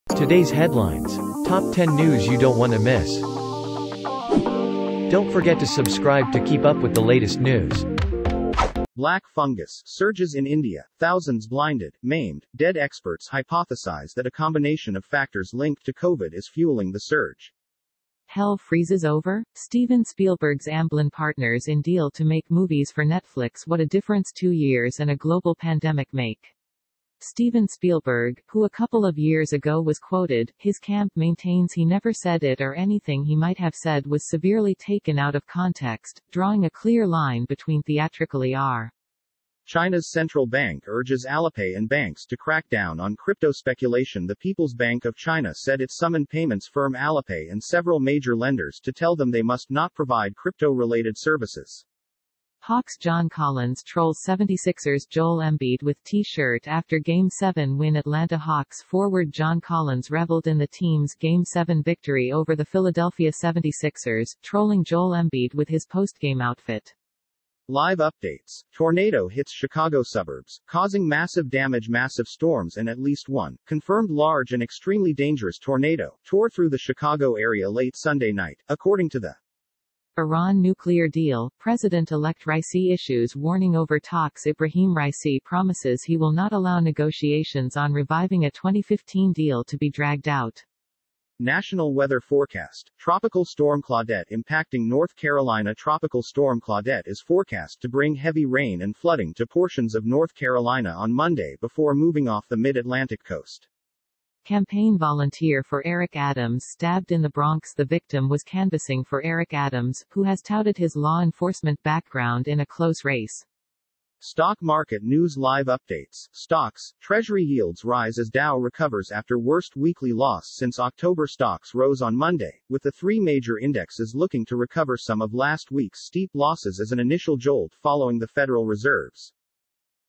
BLACK FUNGUS - TODAY'S HEADLINES
In this video, you will find the top 10 headlines of the day.

00:00 - Intro
00:10 - Black fungus
00:19 - Steven spielberg ’s amblin partners
00:40 - China central bank urges alipay
00:52 - Hawks' john collins trolls
01:07 - Tornado hits chicago suburbs
01:19 - President-elect raisi issues warning
01:30 - Tropical storm claudette impacting north
01:43 - Campaign volunteer for eric adams
01:52 - Treasury yields rise as dow